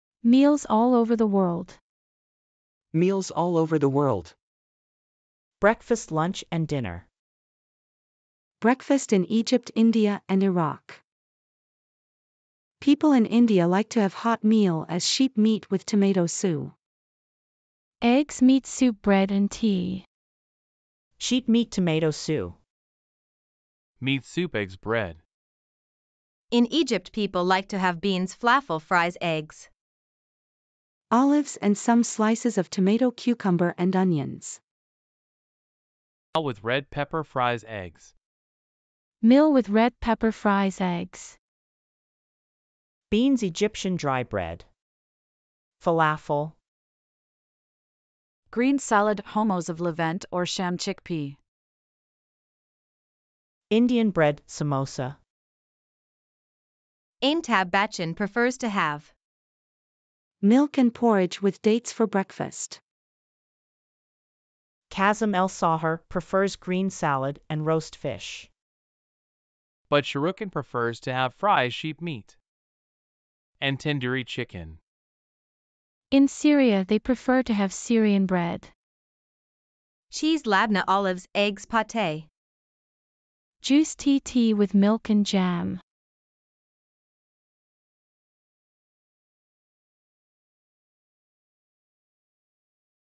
Meals allover the world .
In this video you will know different kinds of food people like to have  at breakfast . In Egypt ,India and Iraq .